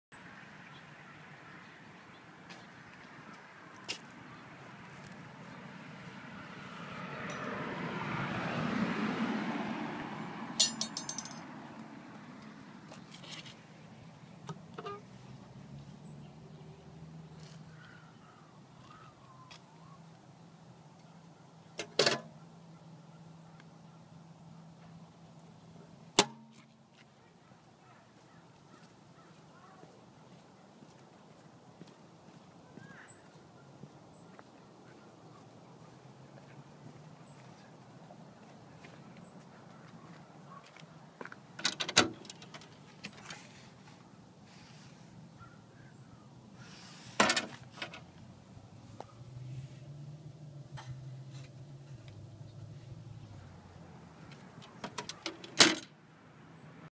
Mailbox ideas
This is an example of a reasonably well-implemented mailbox design.  It opens at both ends, has an outgoing slot and flag, and has a motion-sensing light and magnetic latch.